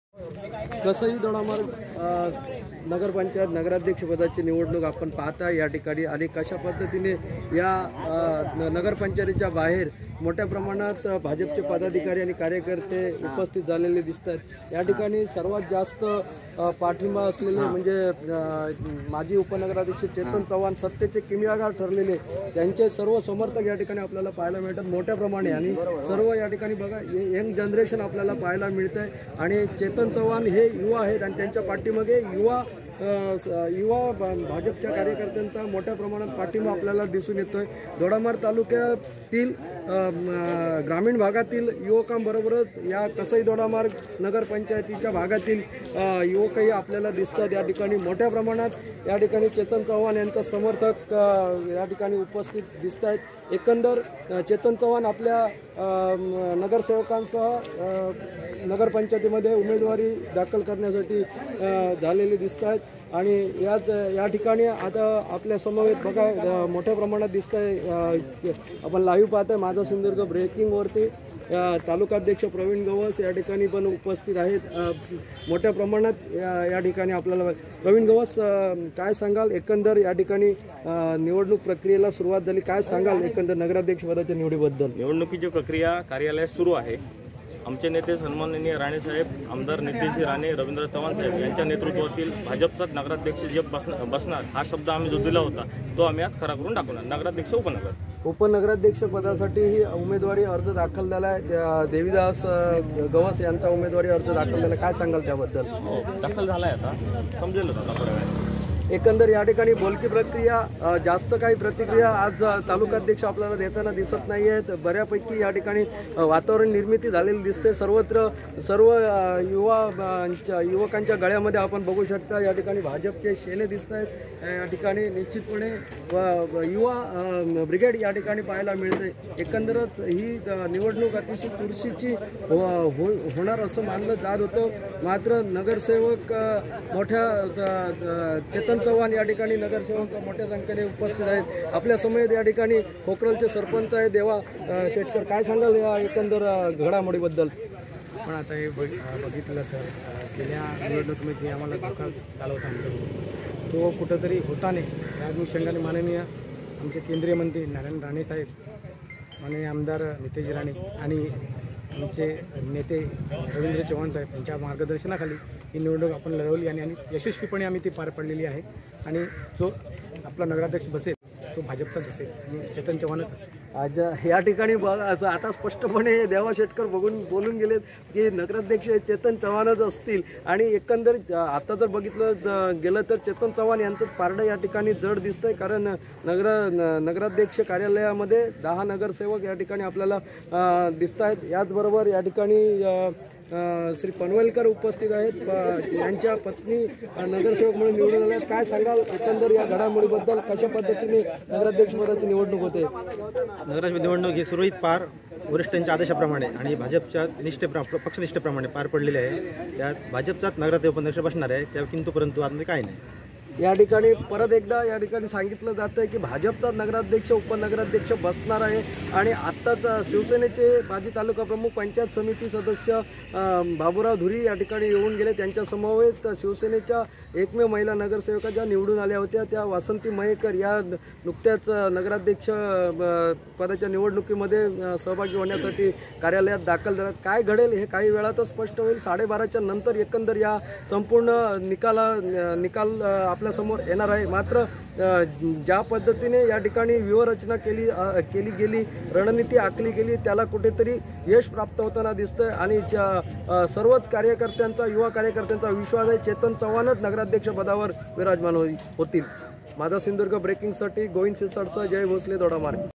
दोडामार्गात चेतन चव्हाण यांचे पारडे जड;कार्यालयाबाहेर युवा कार्यकर्त्यांची गर्दी
माझा सिंधदुर्ग ब्रेकिंग... साद तुमची,साथ आमची